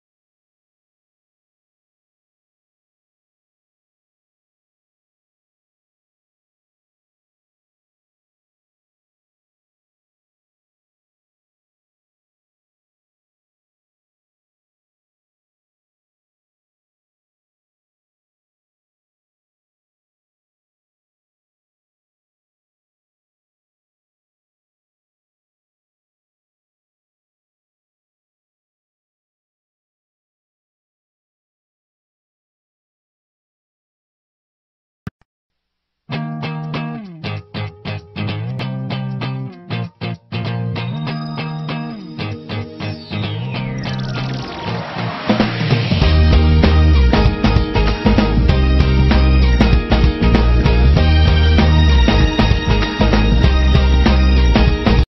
POS PAM Ketupat Toba 2020 dan Check Point di Pos Pam Batu Hula
Pers satlantas polres tapsel dan tim gabungan dari TNI POLRI melaksanakan giat Pos pam ketupat toba 2020 di pos pam batu hula kec. Batang toru. Kab. Tapsel.